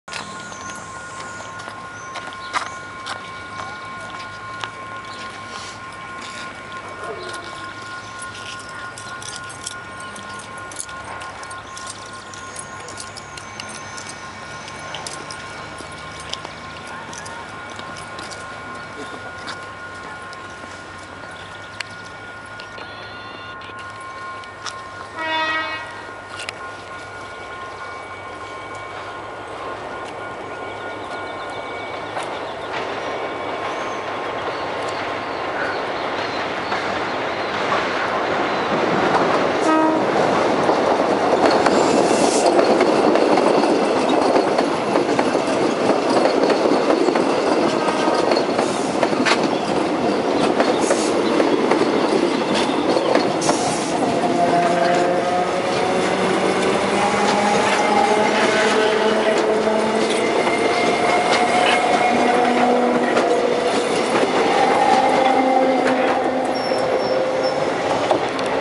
Ferrocarril Urquiza, estacion Martin Coronado 04-9-2013
por problemas en señales, algunos servicios corrìan solo hasta Martin Coronado